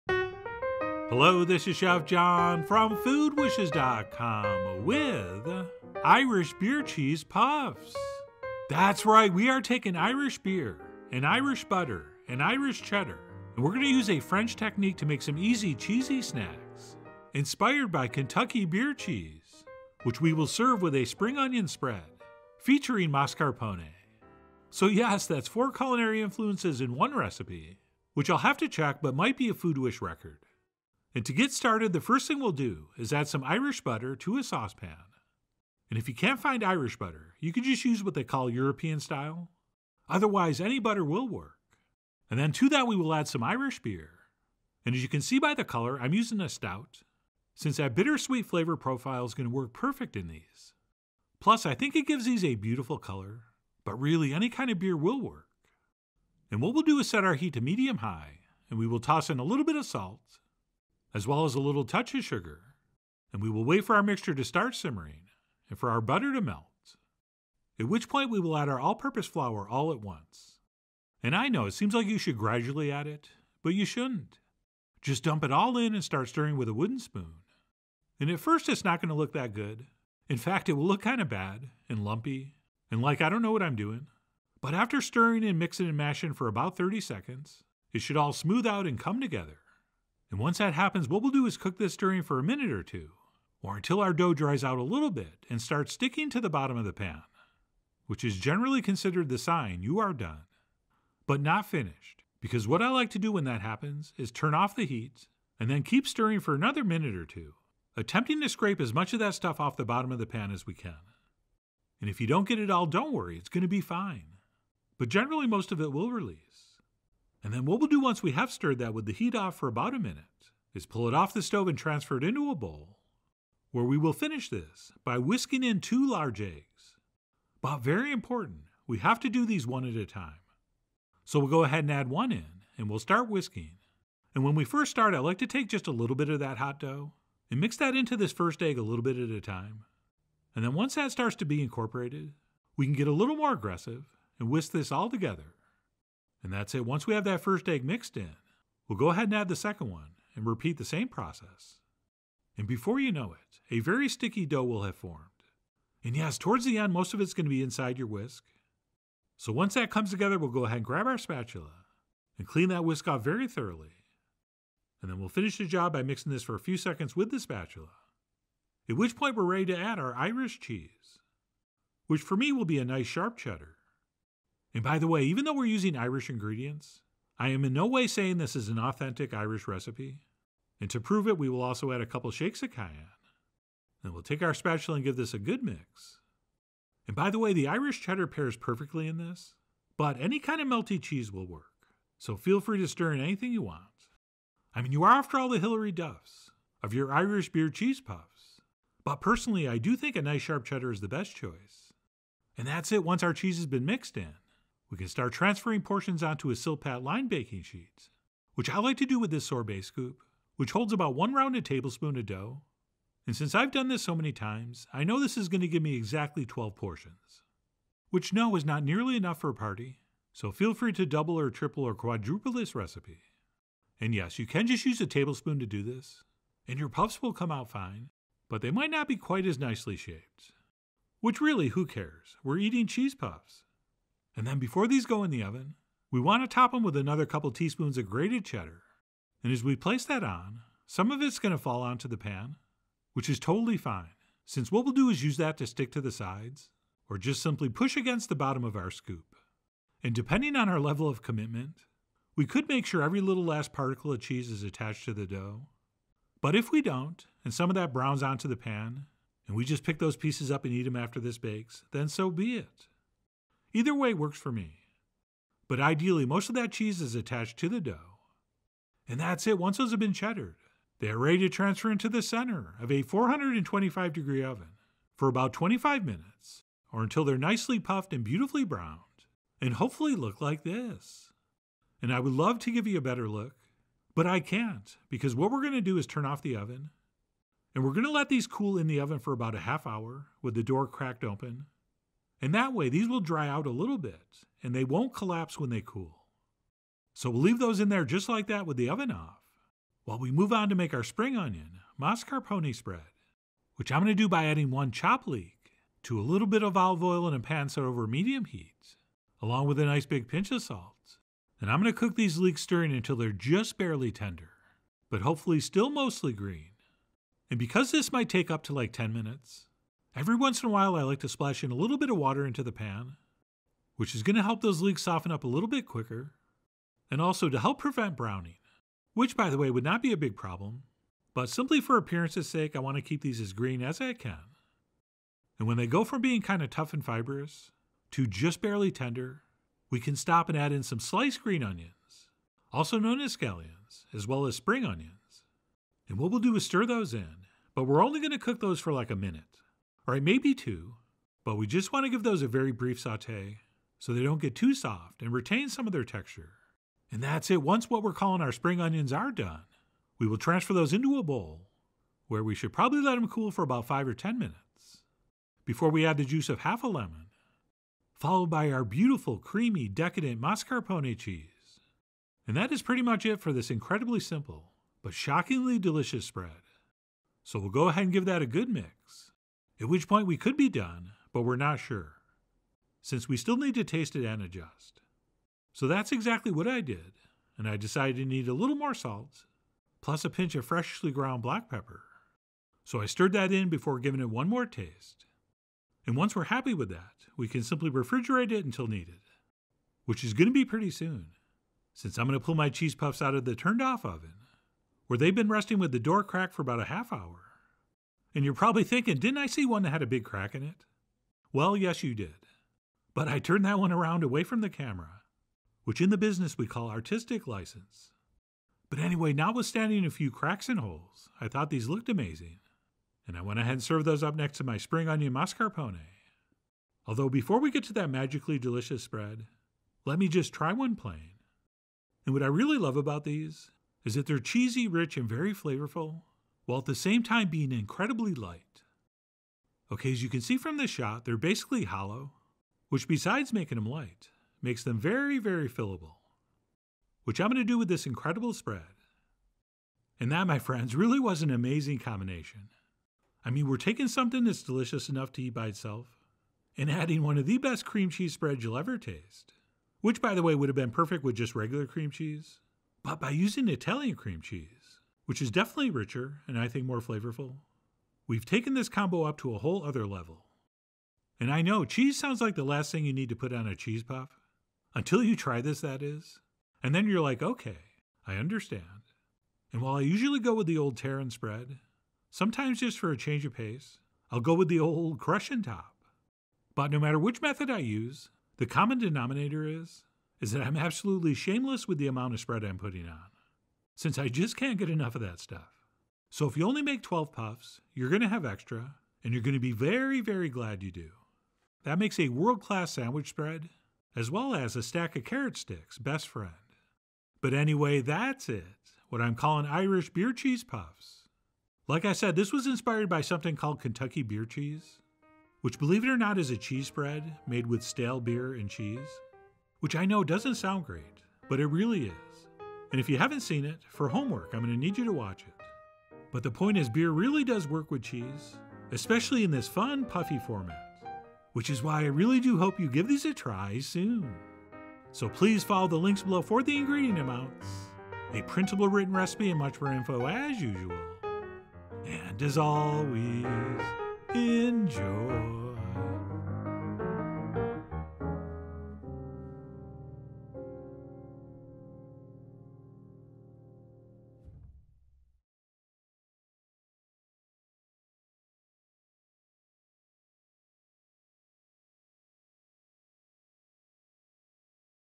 Irish Beer Cheese Puffs with Spring Onion Mascarpone - Food Wishes
While this tasty snack might not be authentically Irish, they feature Irish cheese, Irish beer, and Irish butter, and that’s close enough for me. You can serve them hot, warm, or room temp; hopefully next to the equally delicious spring onion mascarpone spread. Enjoy!

To become a Member of Food Wishes, and read Chef John’s in-depth article about Irish Beer Cheese Puffs, follow this